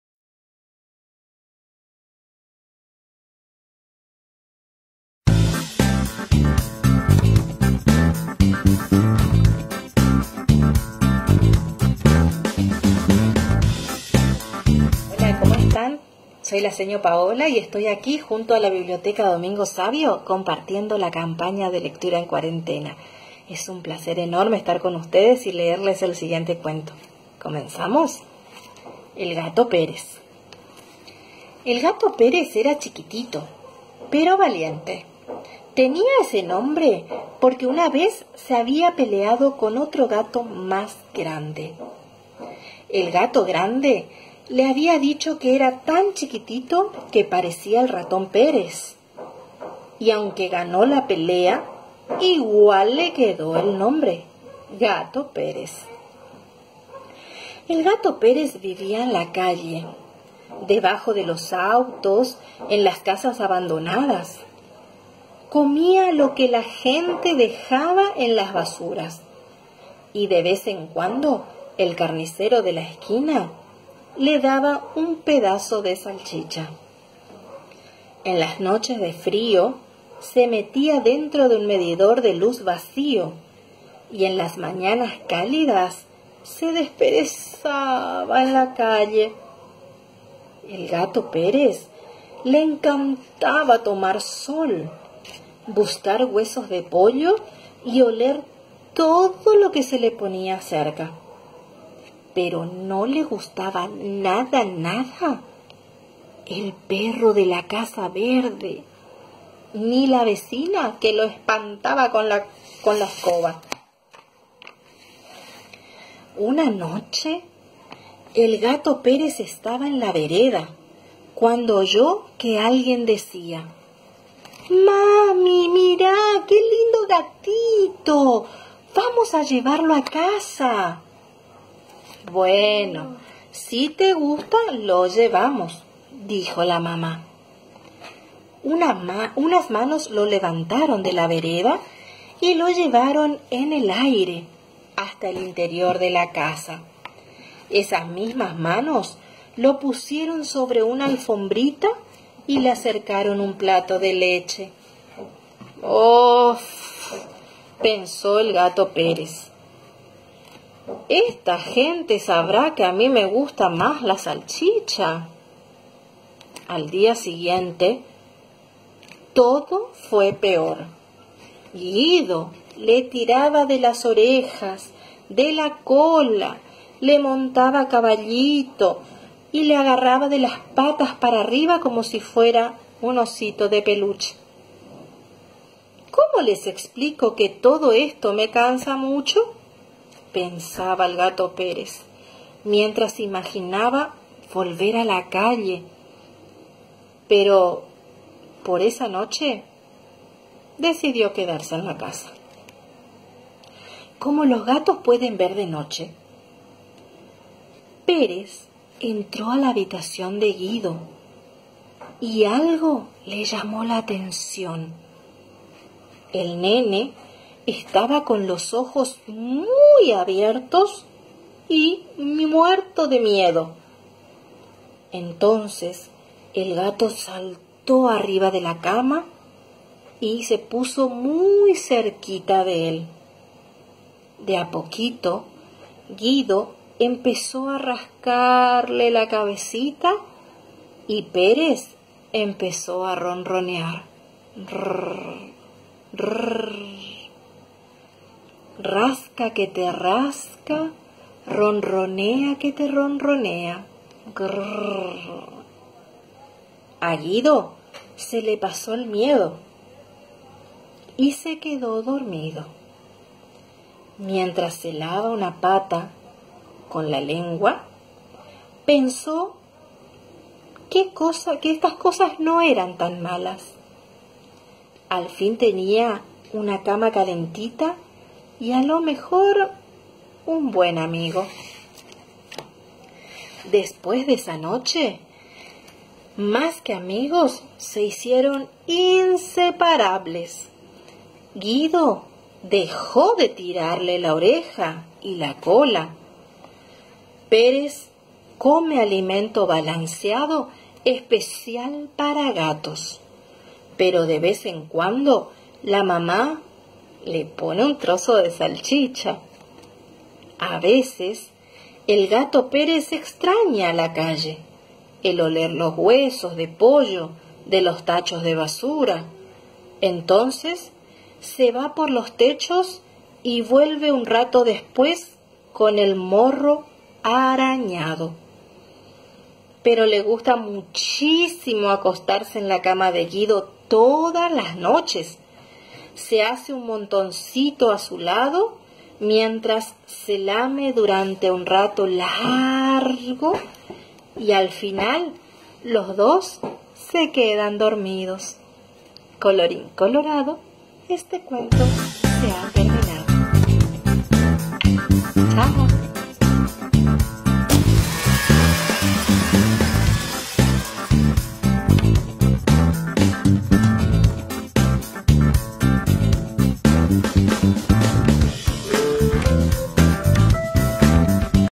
BP N° 236 Domingo Savio. El gato Pérez
Biblioteca Escolar Popular N° 236 "Domingo Savio"

Este Jueves tenemos el agrado de que nos cuente un cuento la Seño Paola Mancuello de la E.E.P Nº 168, muchas gracias Seño!